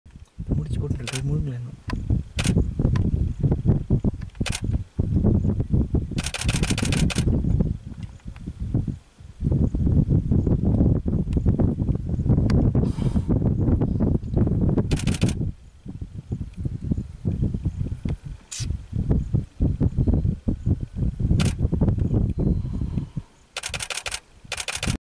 Short-toed Snake Eagle swallowing a Spectacled Cobra !!
This STSE was initially sighted perching on the ground. After a brief preening, it took to the wing and started hovering 15-20 metres above the ground. Suddenly, the eagle started to glide away from us quickly and was descending very slowly towards thick grass. It glided at least 500 metres from us and landed next to a dry grass patch. A few minutes of observation revealed a large snake struggling in its talons. We carefully approached the eagle to find a full grown Spectacled Cobra Naja naja struggling for its life. Quickly the eagle pecked the snake's head, gave a powerful bite and started swallowing the snake whole. It took just 2 minutes to consume the entire snake.

The true power of Snake Lord was hence proved !!